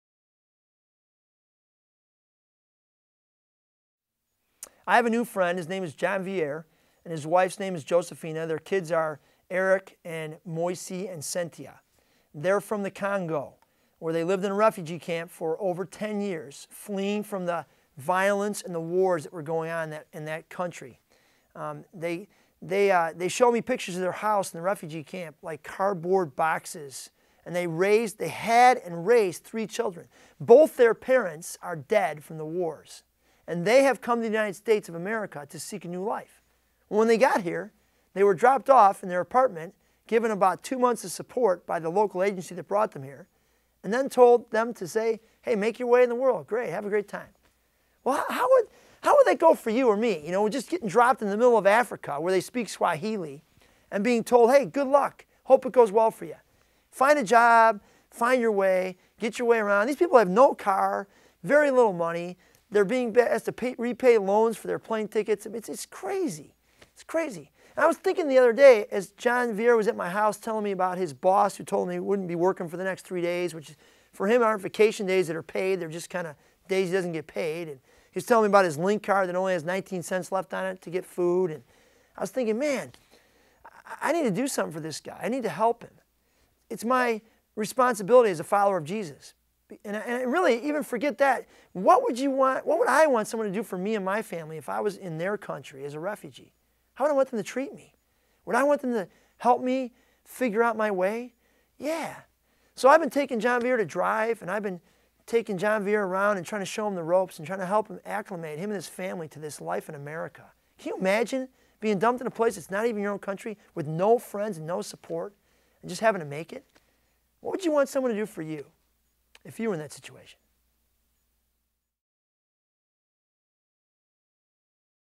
Walk The Way Episode 230: Do Unto Others
It can be easy to ignore injustice, to ignore those who need our compassion. It is easy to make excuses. But if we truly do unto others...